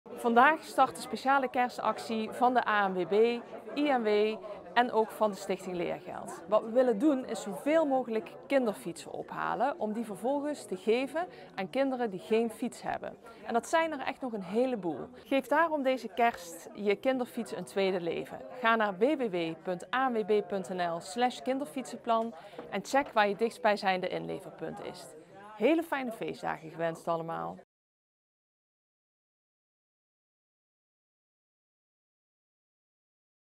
Aftrap kerstspecial Da's zo gefietst met staatssecretaris Vivianne Heijnen
Staatssecretaris Vivianne Heijnen gaf vandaag het startschot voor de kerstspecial van de campagne Da’s zo gefietst. Bij deze campagne roepen we Nederlanders op om hun niet meer gebruikte kinderfietsen in te leveren. Deze worden opgeknapt en geschonken aan kinderen die anders geen fiets kunnen krijgen.

Een groot deel van de kinderfietsen wordt (bijna) niet gebruikt. Daar kan verandering in komen. Mensen kunnen hun gebruikte kinderfiets inleveren bij ANWB-inleverpunten. De gemeenten zorgen dat deze hashtag#fietsen goed terechtkomen.

Help je ook mee om meer kinderen van een goede fiets te voorzien?